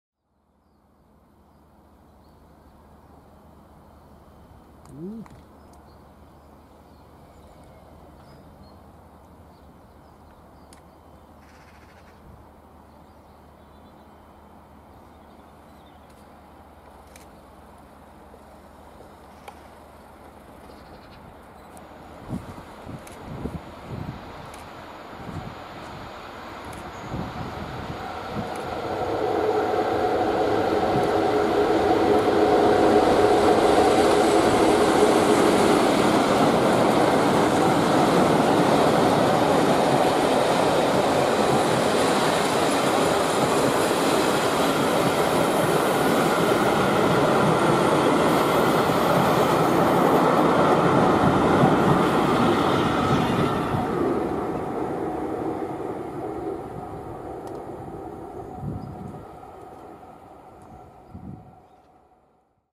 47805 (D1935)/47501 (D1944): 1Z56 11:00 Kingussie - Crewe | Coatbridge (25/03/18)
LSL Private Charter heads back home south with their double-heided green duffs, seen coasting just outside Coatbridge Central station with the sun out and about.